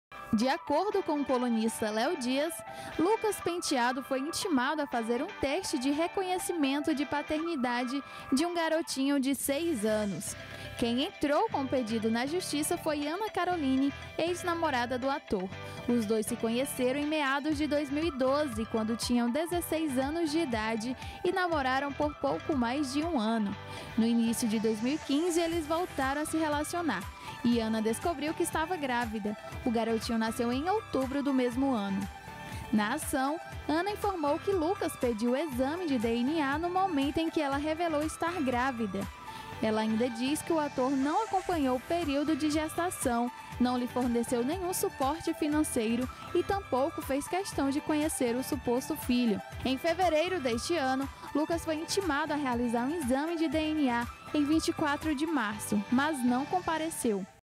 Lucas Penteado é intimado a fazer teste de DNA por filho rejeitado
Lucas Penteado foi intimado a fazer um teste de reconhecimento de paternidade de um garotinho de seis anos. Quem entrou com o pedido na Justiça foi Ana Karoline Acre Ferreira de Sousa do Nascimento, ex-namorada do ator.